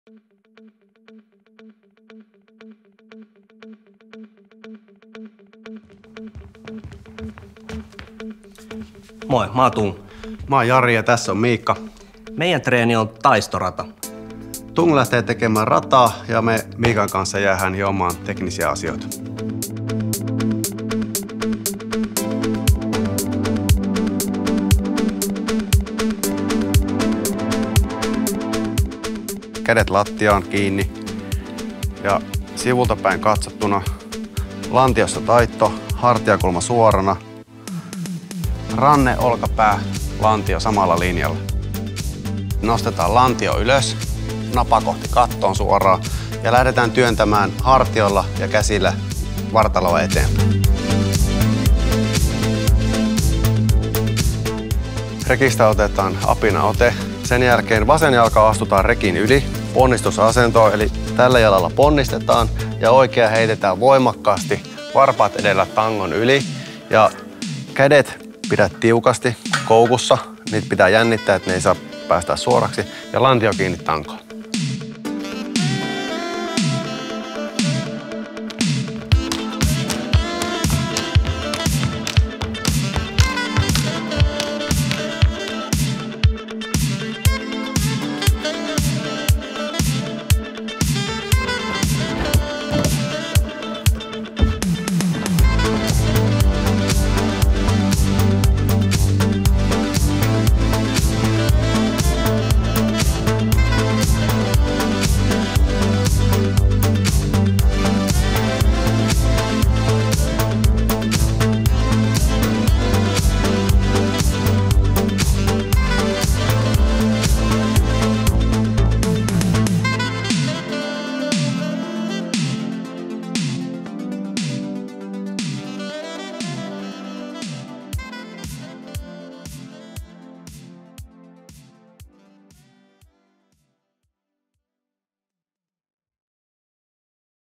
Taisto Workout 1 - Jari Mönkkösen ja Tung Buin ratatreeni
Esimerkkitreeni: (soveltaminen on sallittua!)

Alkuun 10 min. lämmittely oman mielen mukaan; esimerkiksi juoksua tai pelailua, kuten jalkapalloa salissa. Kevyt venyttely.

Ratatreeni suoritetaan aikaa vastaan. 1 kierros, jossa eri haasteet ovat seuraavassa järjestyksessä:

1. Käsillä kävely vatsa seinää vasten. Edetään sivuttain 5m.

2. Voimakävely 5m liukkaalla alustalla tai liukkaalla esineellä. Vartalo on jännitettynä käsien etupuolella.  Työnnetään vartaloa eteenpäin.
Helpotettu versio: Rapukävely 5m eteenpäin, eli jalkapohjat ja kämmenet maassa kävely. Lantio ylhäällä.

3. Kieppi tai juoksukieppi rekillä x 3
Helpotetut versiot: Ponnistus matalan pukin päältä tai kieppi avustajan kanssa.

4. Parkour esteet: 2 x esteen ylitys omalla tyylillä.

5. Naruhyppely pujotellen n. 5m, 4 törppöä eteen + taakse.
Helpotettu versio: ilman törppöjä hypitään 5m eteen + taakse.
Helpotettu versio: hypitään paikallaan 30 sekuntia.

6. Parkour catleap: Hyppy roikuntaan (jalat "muurille") josta polvi edellä 1/2 käännös pukin päälle hallitusti ja tasapainossa + "rolli" kuperkeikalla eteenpäin.
Helpotettu versio: Hyppy maahan ilman pukkia + rolli.

7. Matolla kuperkeikka eteenpäin + 1 1/2 käännöstä x 4 + heti perään tarkkuushypyt luistamattomien muotojen päällä (9 muotoa maassa).

Seuraa Instagramissa: @taistochallenge